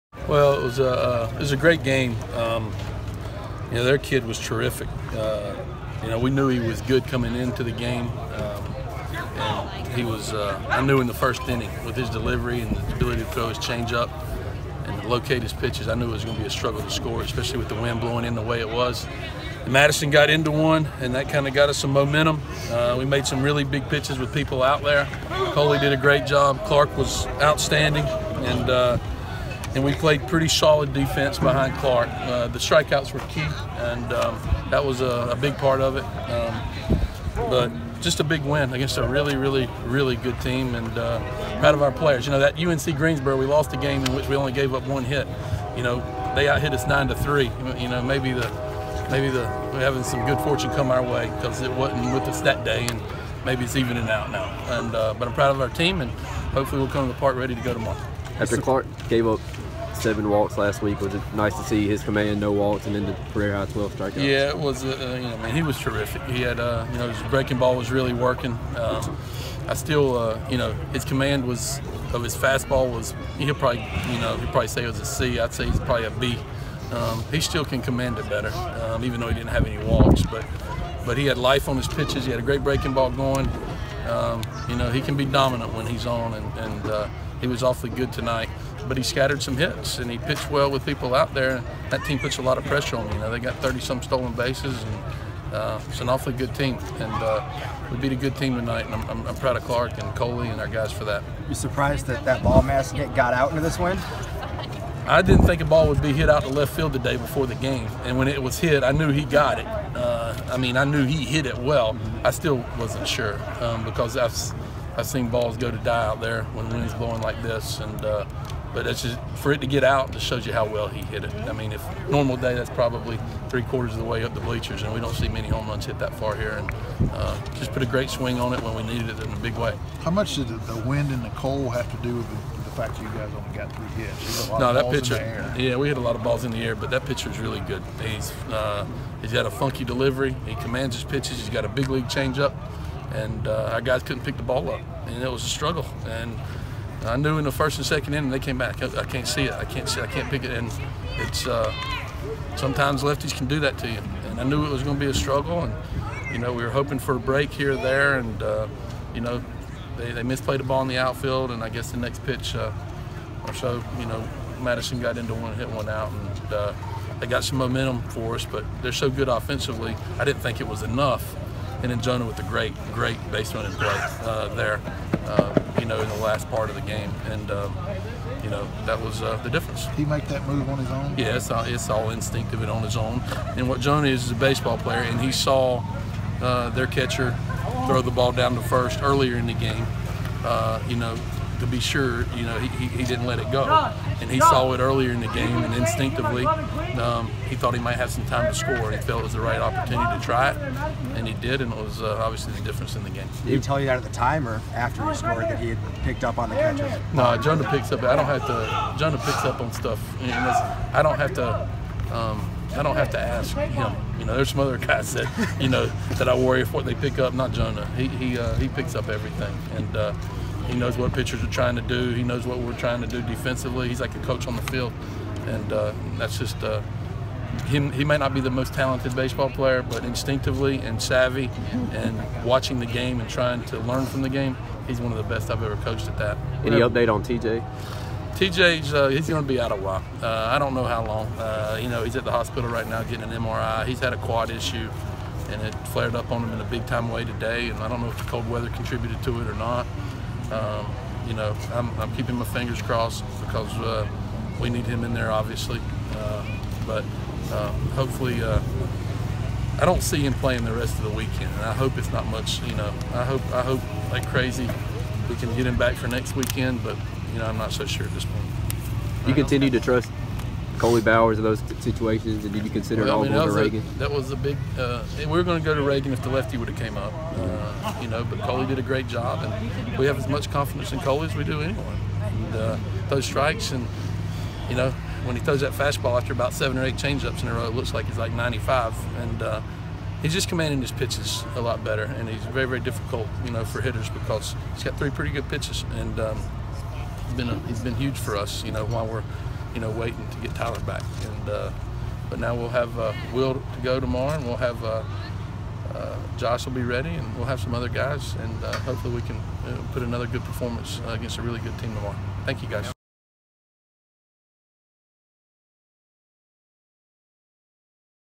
Chad Holbrook recaps USC's win over Michigan State
Gamecocks top Spartans 3-2 in Game 1